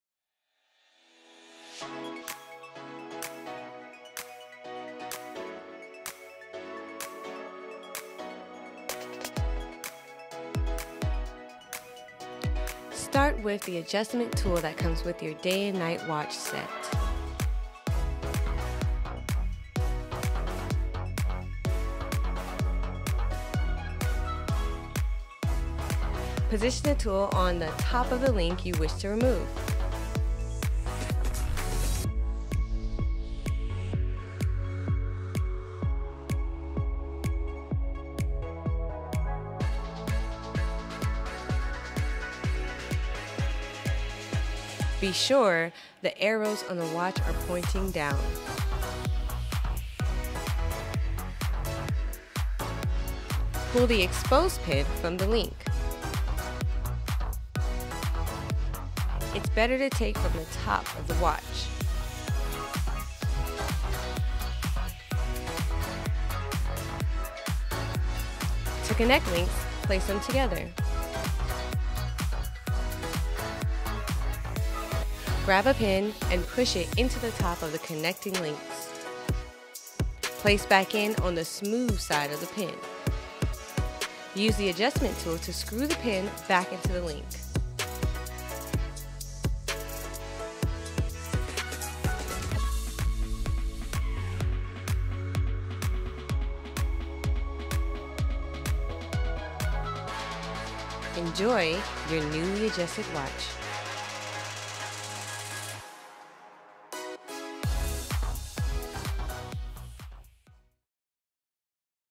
How To Adjust The Links on Your Watch
In this video, we will show you how to resize the watch band by removing the links. With the proper tool we offer with each watch purchase, the steps are very simple. Follow the step-by-step in this video.